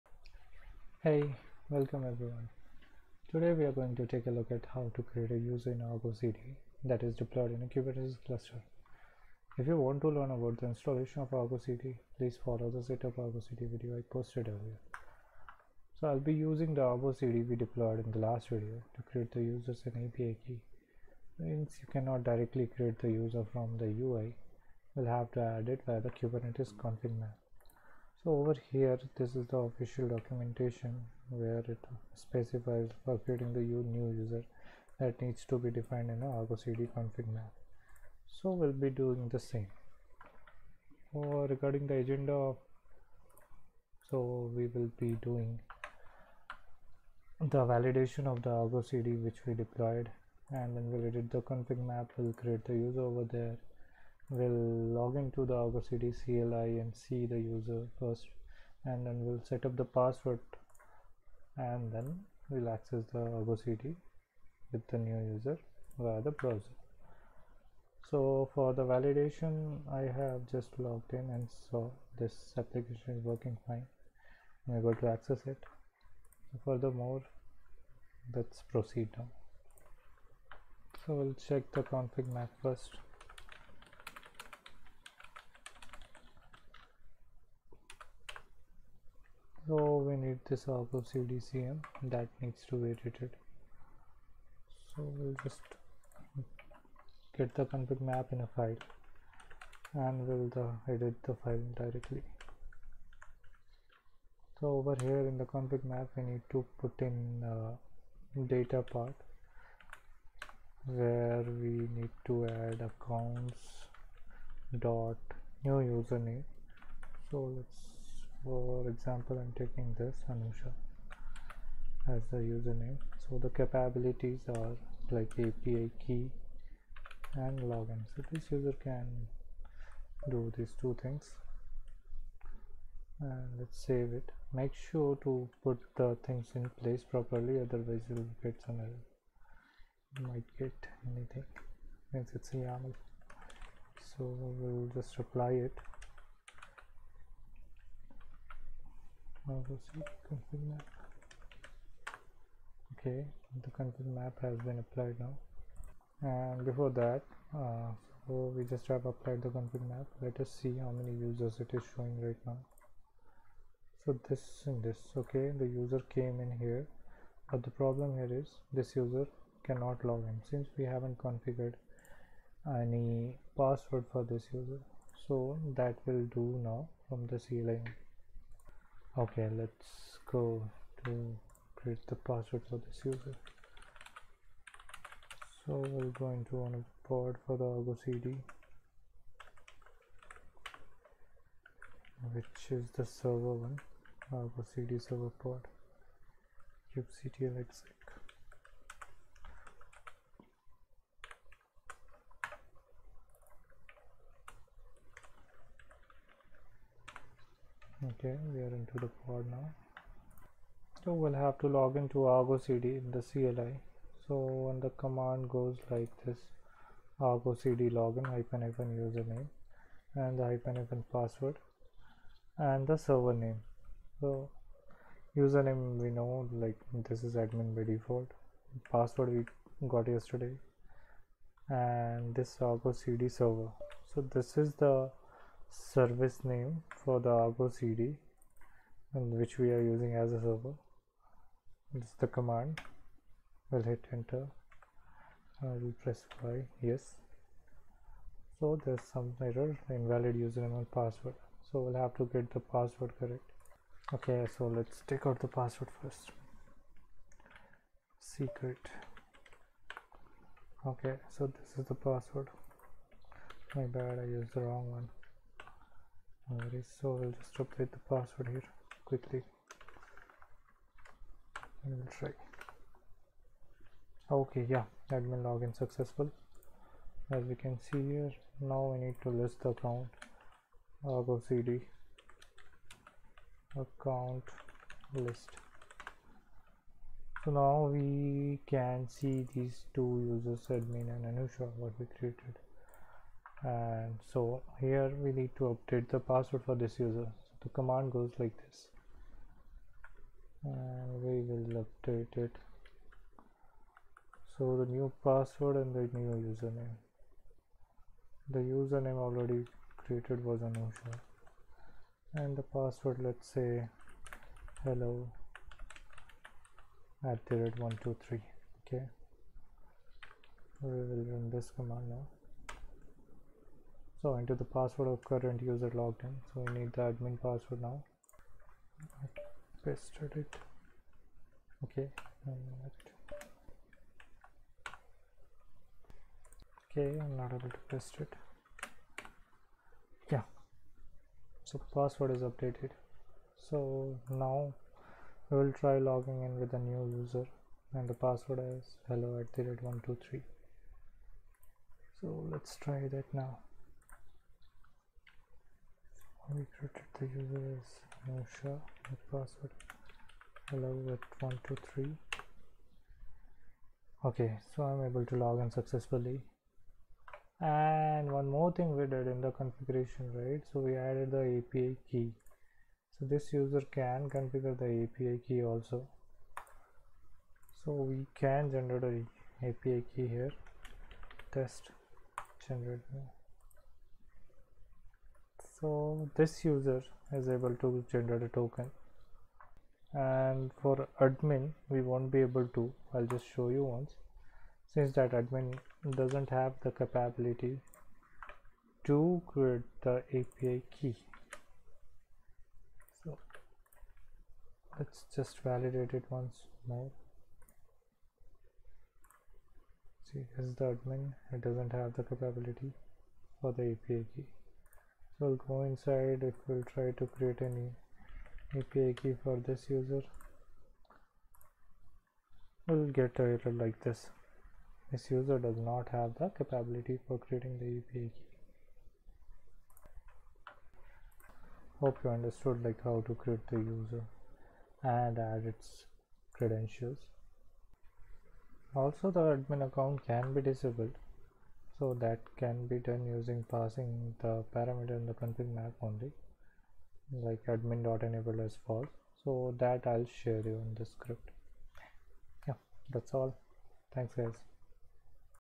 Create user and set password in argoCD
ArgoCD User creation & Password setup

Agenda:
- Validate argocd reachability in browser
- Edit configmap
- Create user
- Login to argoCD CLI via pod
- Setup password for the new user
- Access the argocd with new user